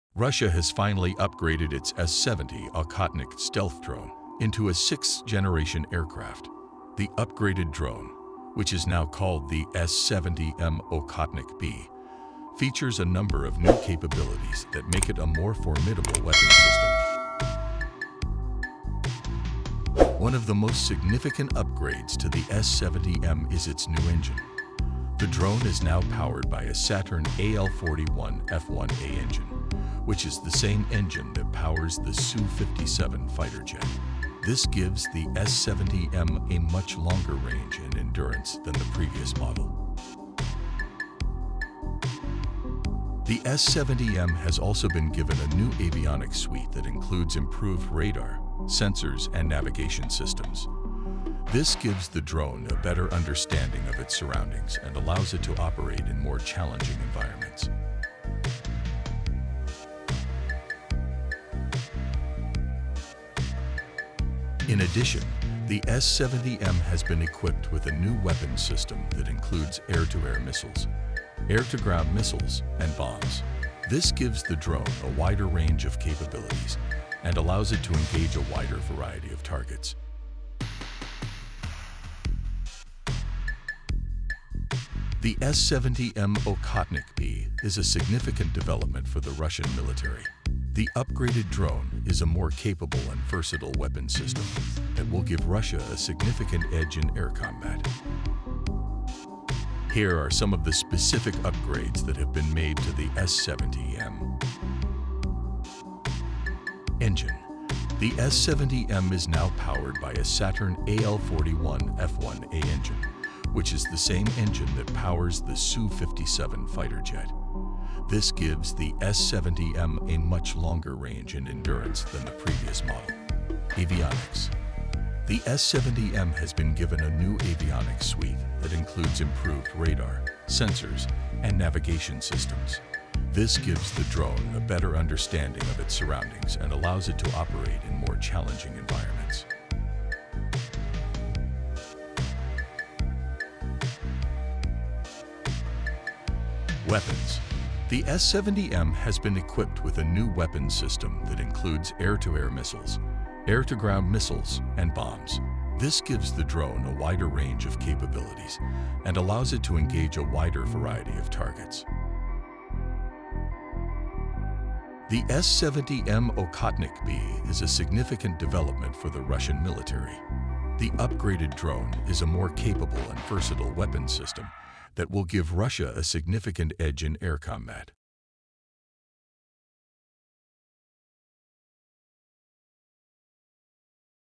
Finally: Russia Upgrades S-70 Okhotnik Into 6th-Gen Stealth Drone
Russia has finally upgraded its S-70 Okhotnik stealth drone into a 6th-generation aircraft. The upgraded drone, which is now called the S-70M Okhotnik-B, features a number of new capabilities that make it a more formidable weapon system.